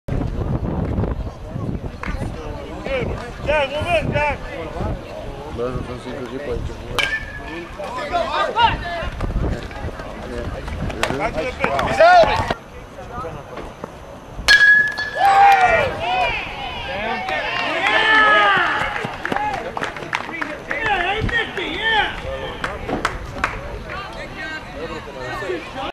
July 4 Game Batting
A Couple of Nicky's at Bats from the July 4 game.